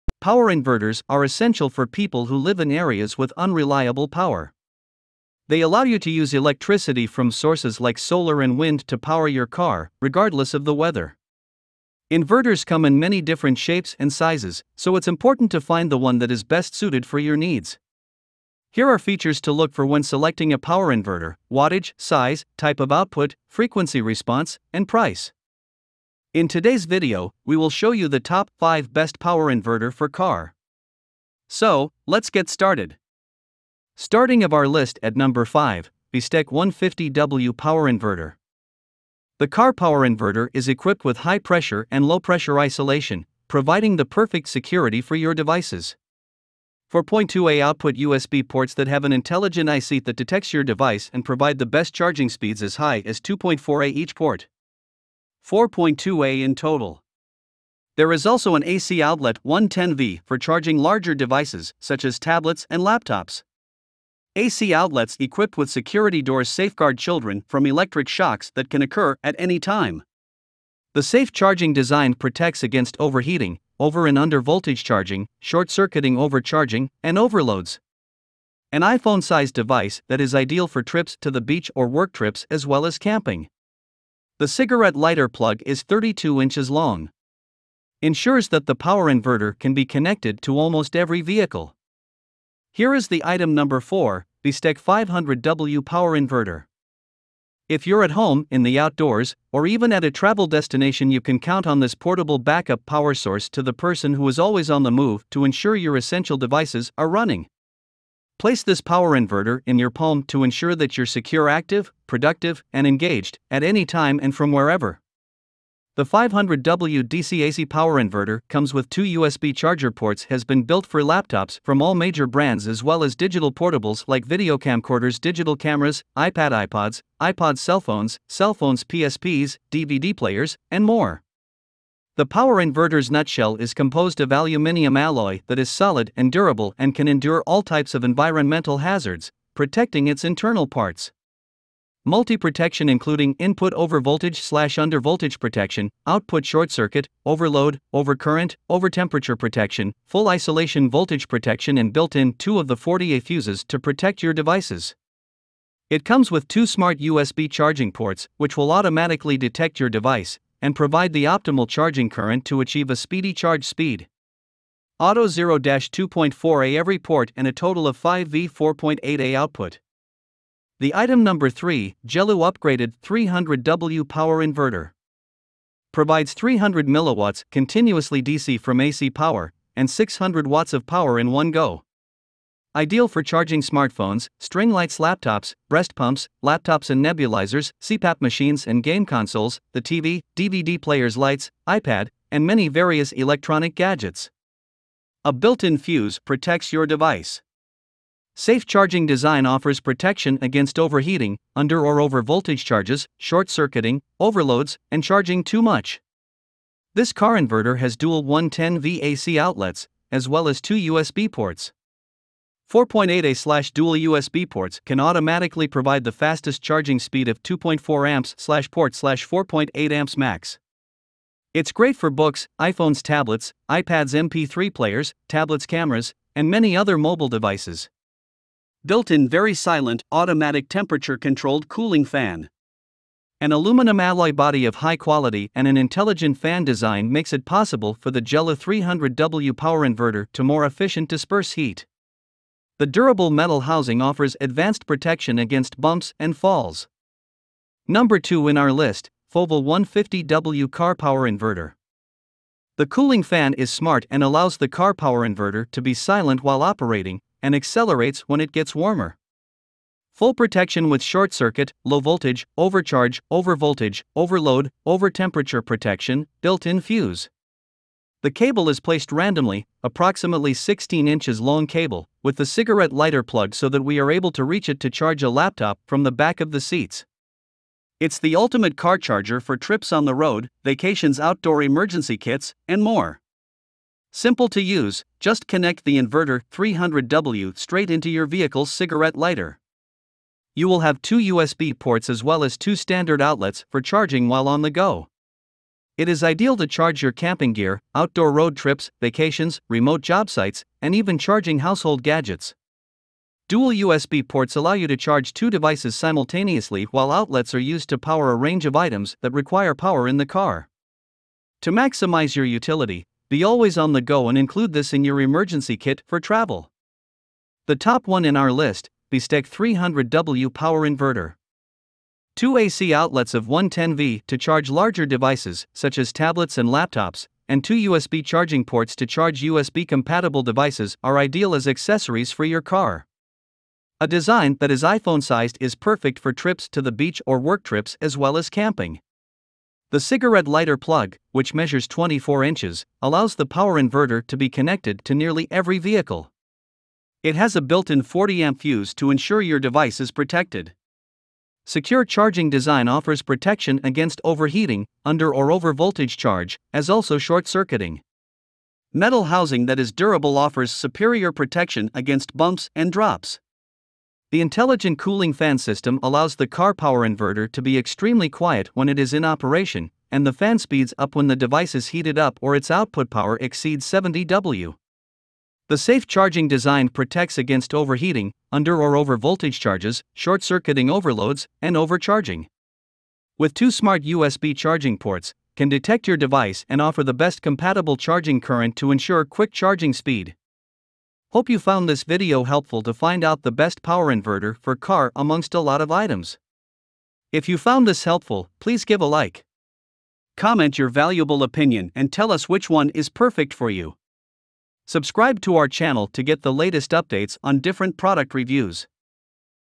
Top 5 Best Power Inverter for Car
5. BESTEK 150W Power Inverter

4. BESTEK 500W Power Inverter

3. GELOO Upgraded 300W Power Inverter

2. FOVAL 150W Car Power Inverter

1. BESTEK 300W Power Inverter